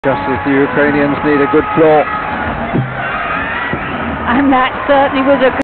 Esther Moya VT1 TF OG 2000
Esther Moya first vault during Team Finals Olympics 2000